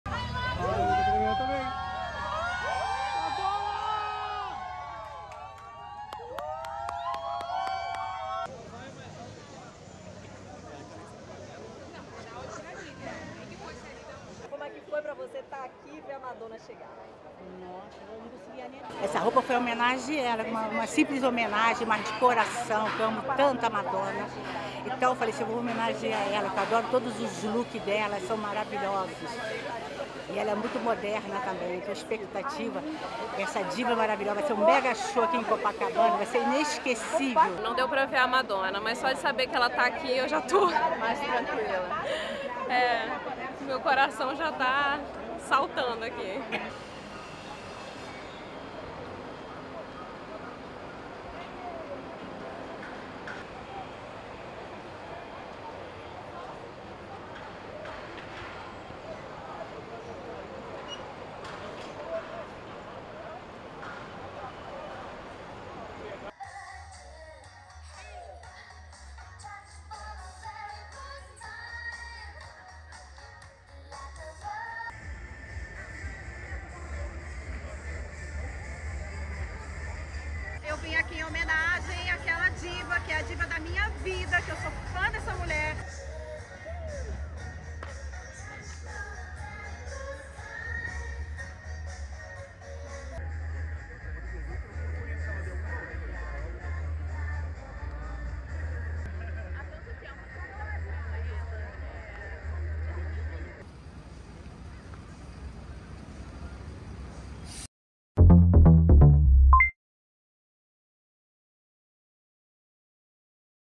BRASIL | El concierto gratuito de Madonna en Copacabana causa furor | EL PAÍS
La emoción de miles de fanáticos de Madonna ha ido en ascenso los días previos a su concierto gratuito en Copacabana, Brasil, programado para este sábado.

La estrella se presentará durante el cierre de su gira ‘Celebration’, que dio inicio en octubre del año pasado. Desde el viernes, cientos de seguidores de la cantante se encuentran acampando afuera del Hotel Copacabana.